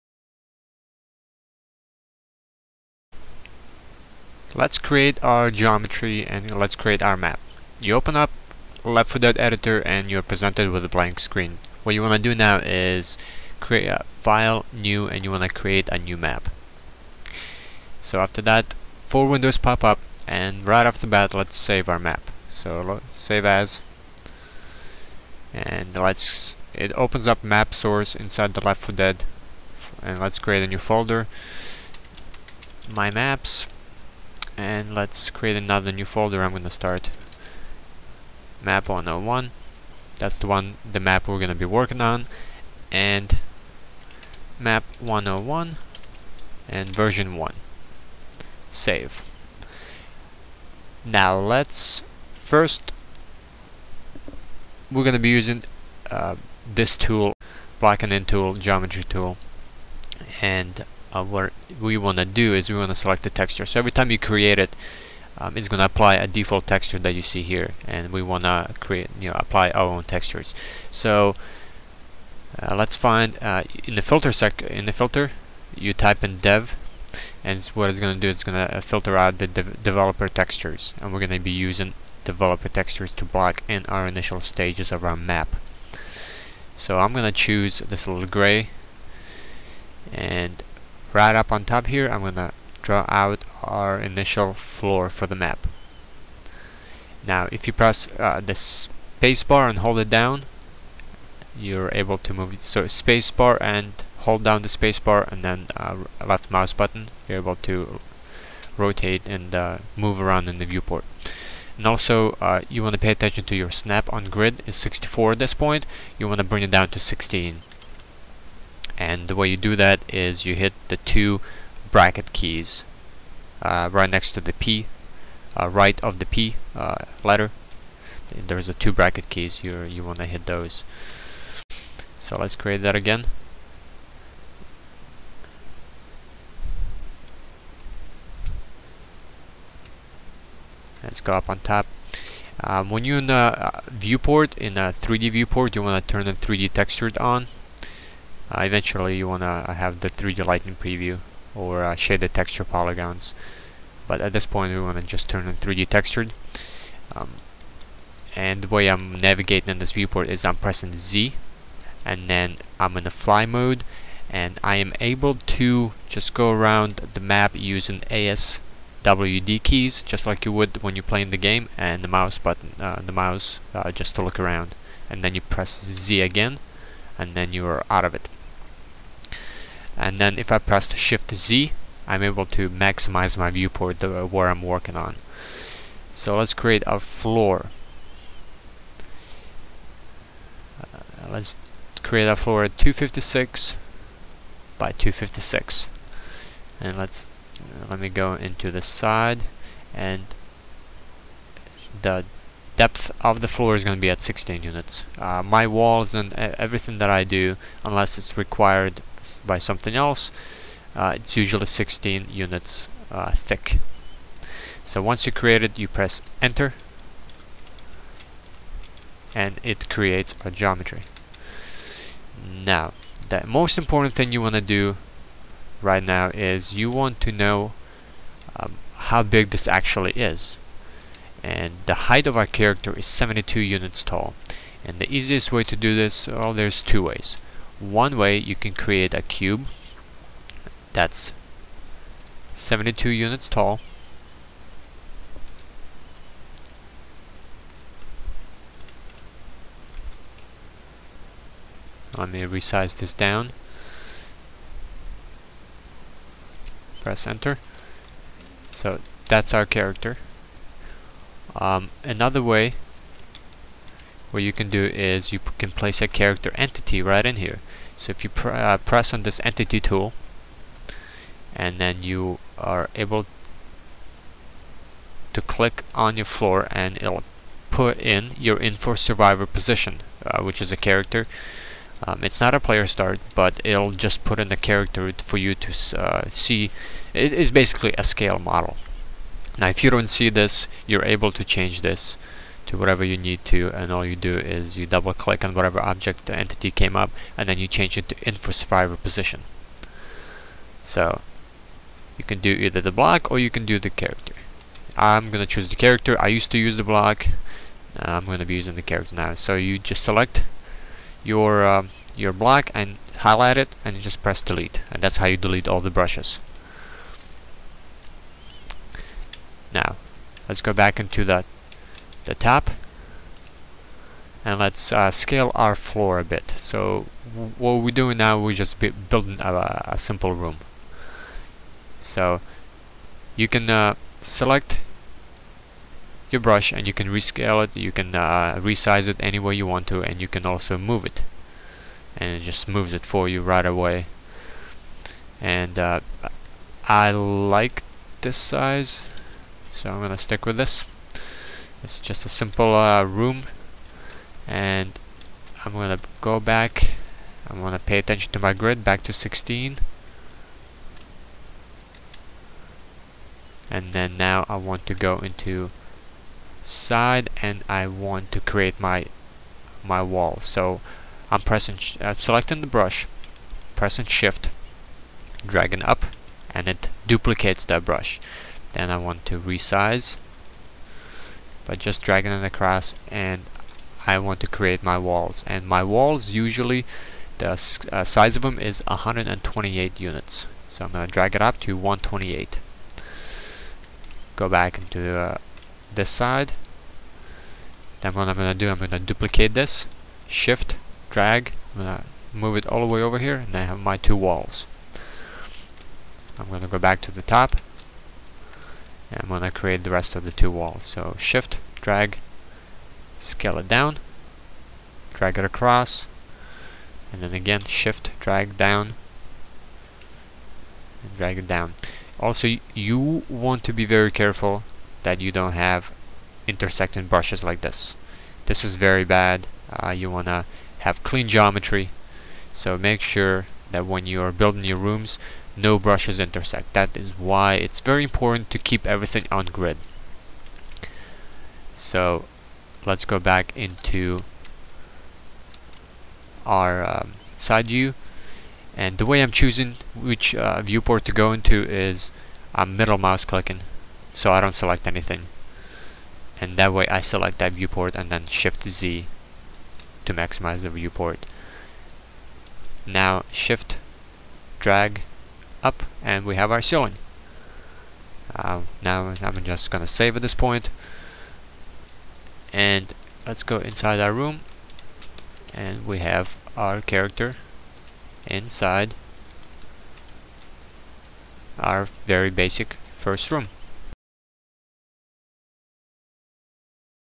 L4D Mapping: Geometry Part 2 - Basic Single Room
In this tutorial we build our first room, and making sure we keep everything to scale.